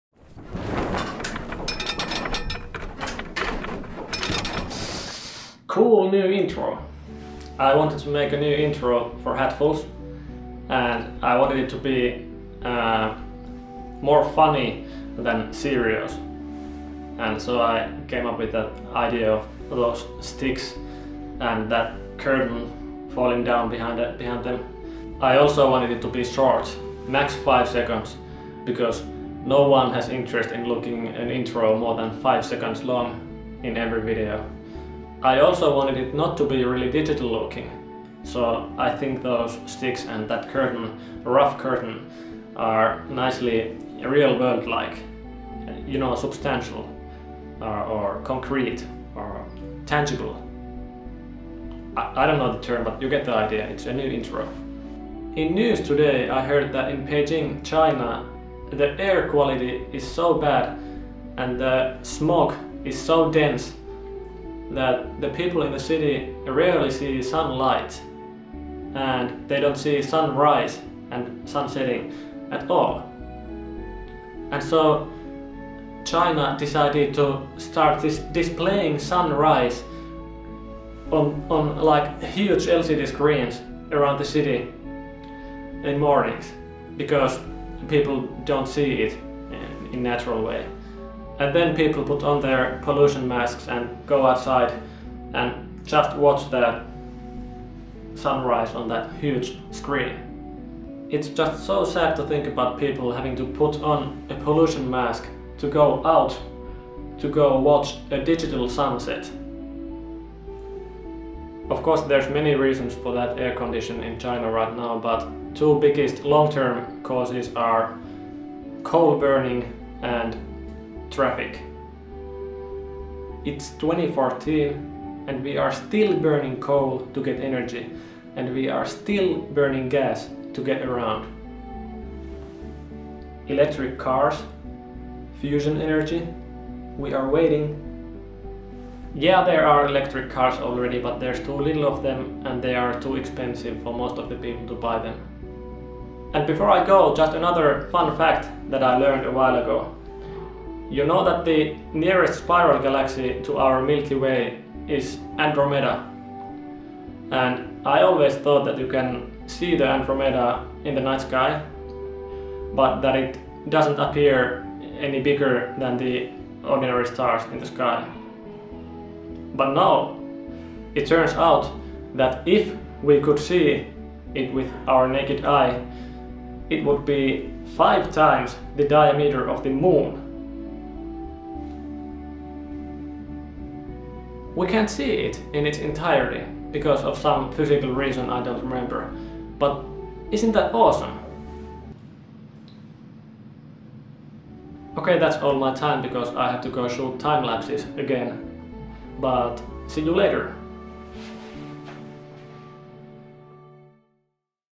New intro!
I made a new intro for hatfuls2.